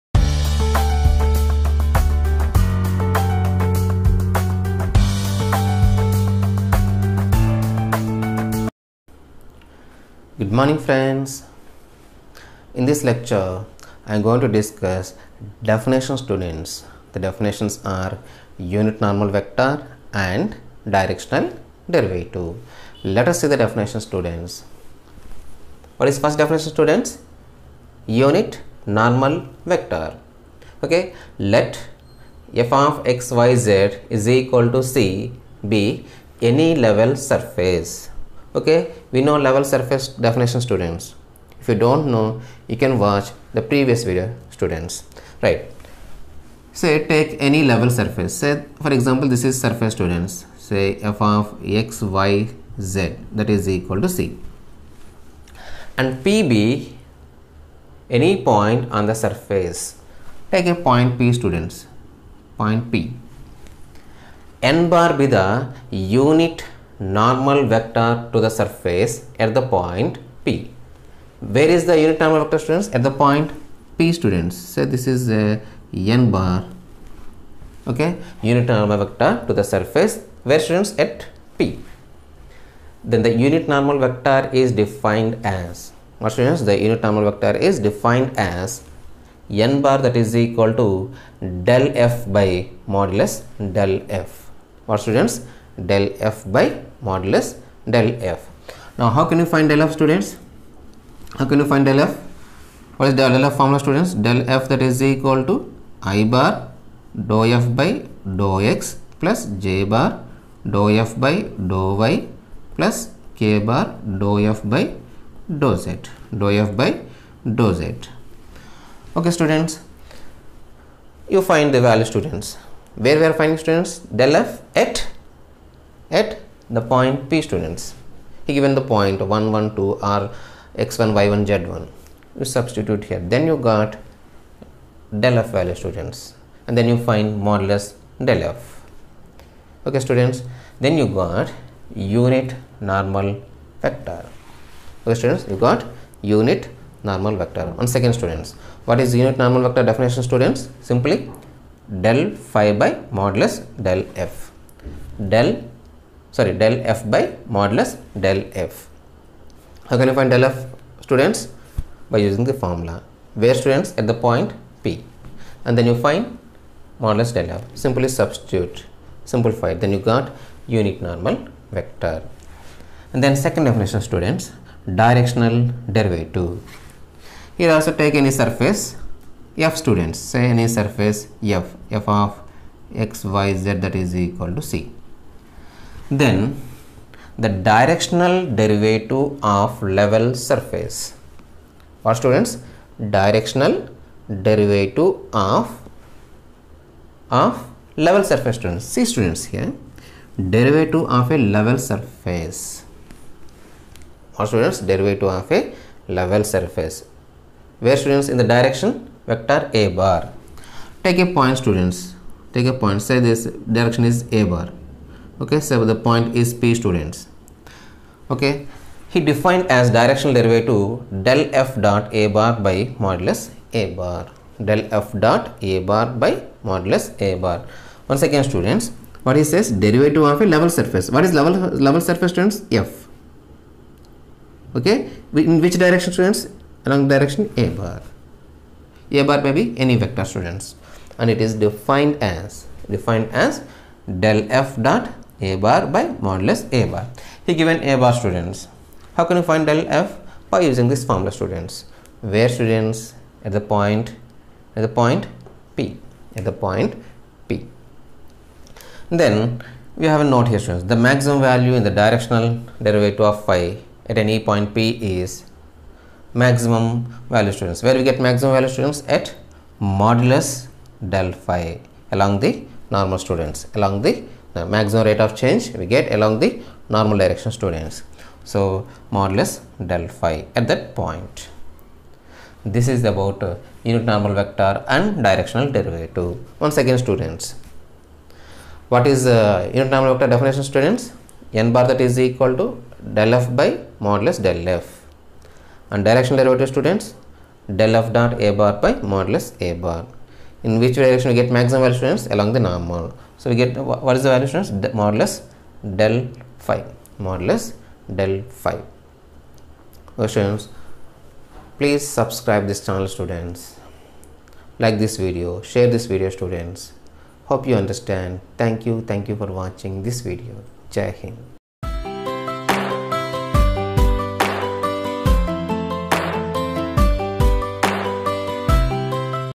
Vector Calculus| Unit Normal Vector, Directional Derivatives| maximum rate of change
I have discussed unit normal vector, directional derivatives and maximum rate of change or value. These topics are very important for understanding the problems.

Sir C R Reddy College of Engineering
Hello friends, today I have discussed  important topic directional derivatives.

vector calculus#vector differentiation
directional derivatives